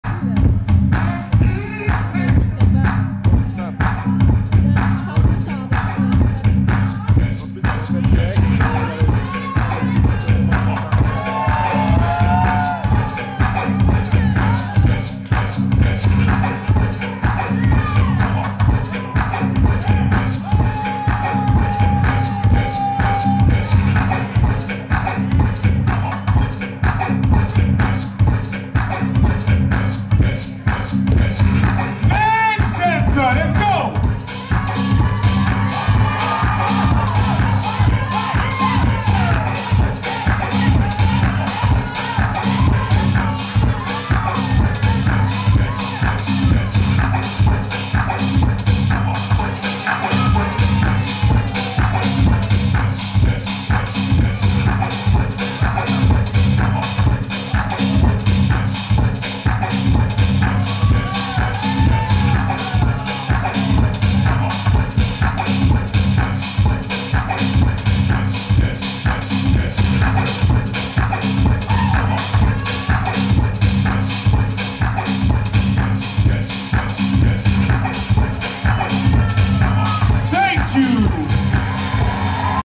Prelim Tie Breaker JAZZ to HOUSE - Princessesi vs. Extra
Tap Water Jam : Jazz to House

Winner Princessesi
28 janvier 2023
Djs Lexis & Kris Guilty
Juge : Kosi
Host : Preach